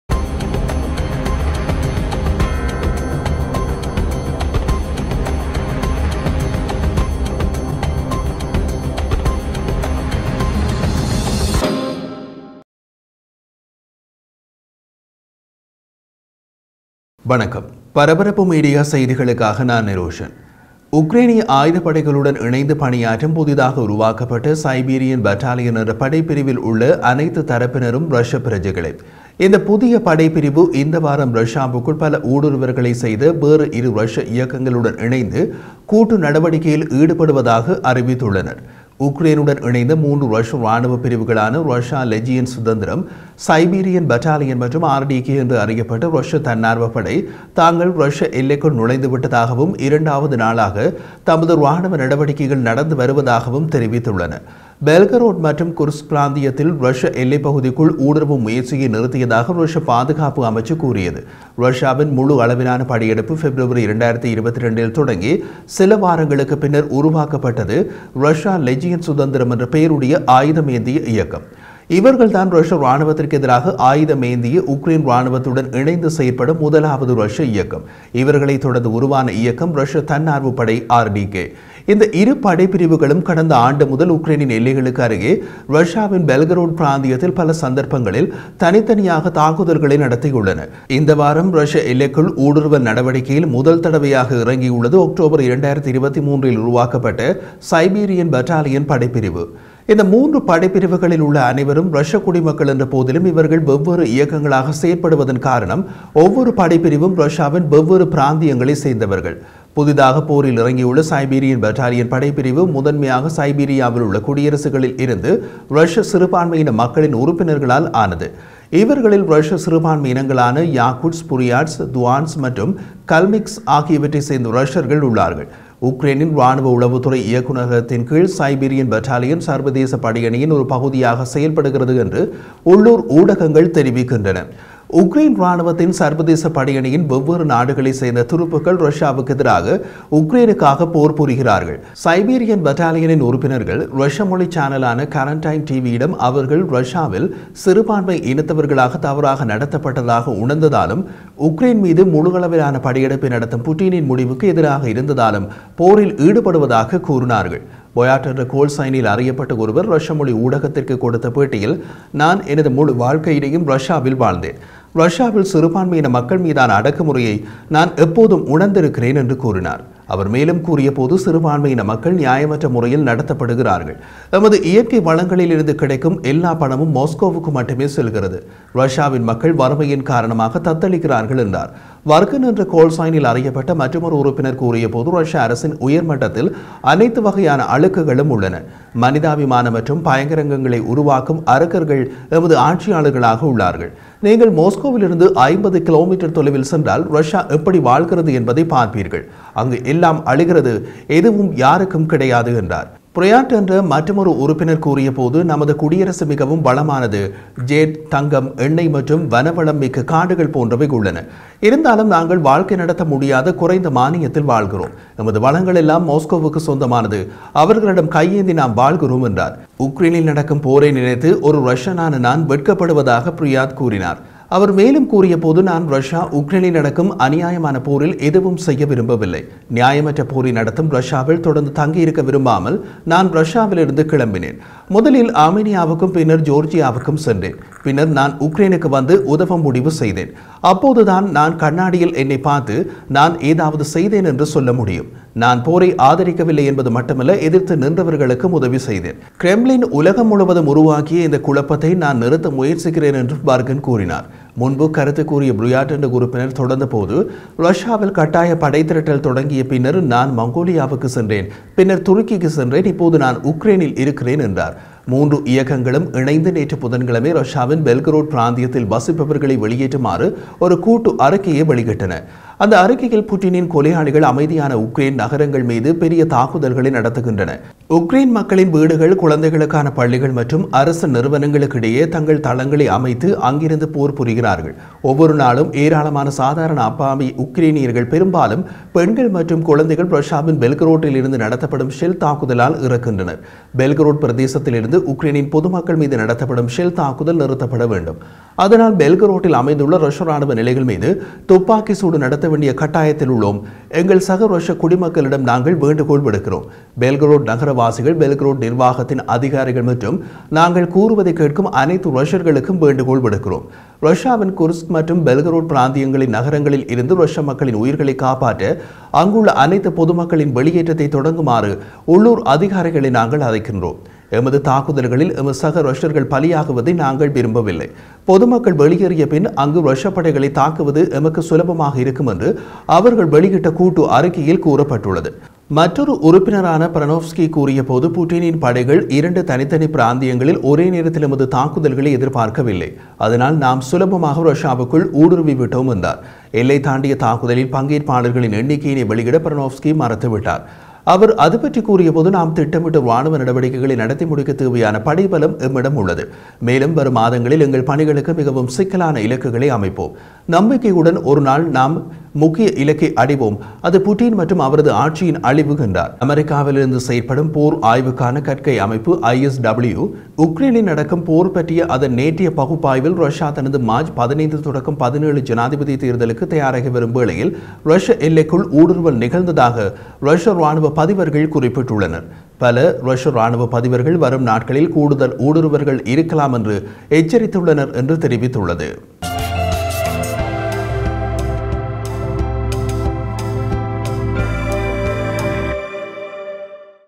ரஷ்ய நகரங்களை காலி செய்ய சைபீரியன் படைப்பிரிவு அறிவிப்பு! | Defense news in Tamil YouTube Channel
All troops in the newly formed Siberian Battalion, which serves alongside the Ukrainian Armed Forces, are Russian nationals. The new brigade announced this week that it would join forces with two other Russian operations that have made several incursions into Russia. Three Russian military units allied with Ukraine, the Russia Legion of Independence, the Siberian Battalion and the Russian Volunteer Corps, known as the RDK, said they had entered Russian territory and were continuing their military operations for a second day.